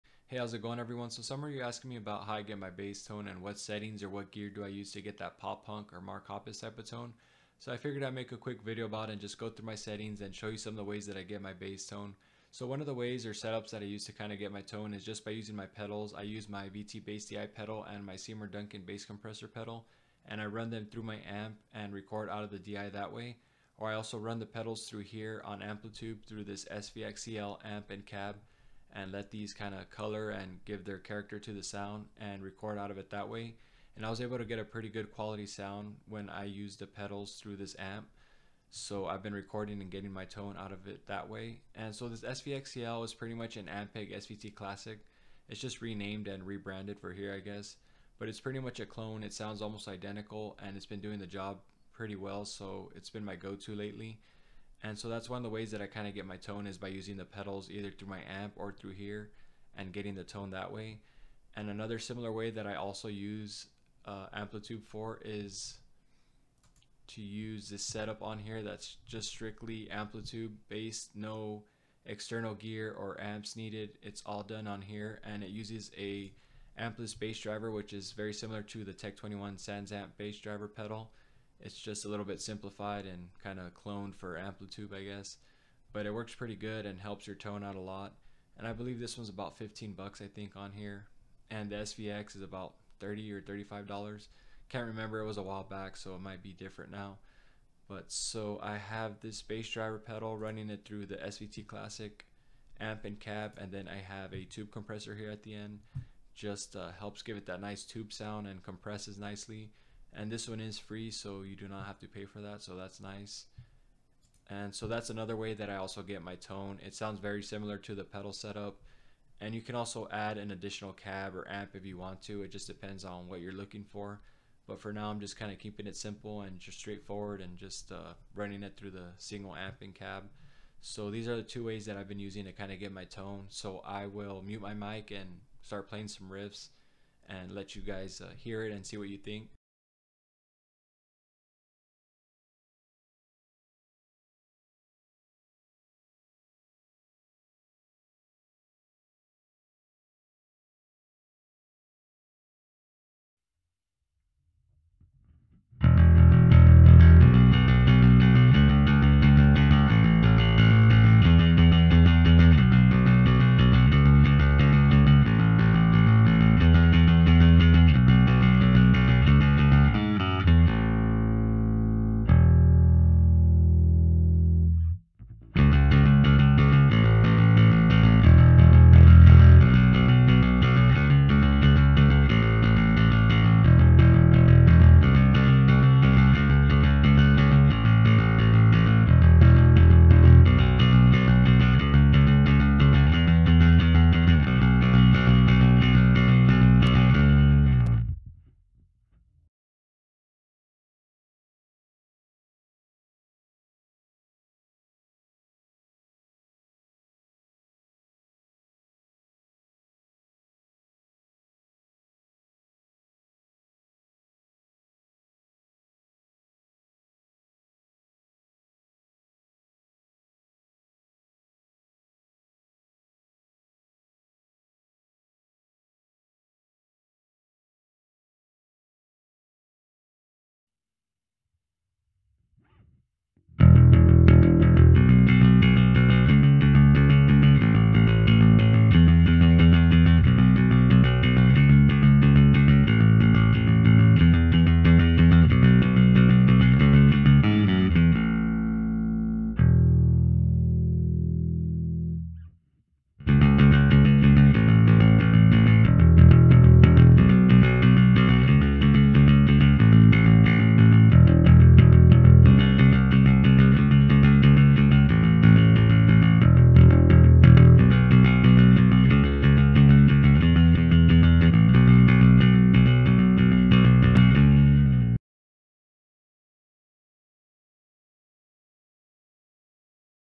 How To Get Pop-Punk/Mark Hoppus Type Bass Tone
Just showing one of the ways I get a dirty pop-punk type bass tone. You can tweak these settings to find something that works for you. Just a quick little tone video using Amplitube if you don't have access to an amp.
-Rick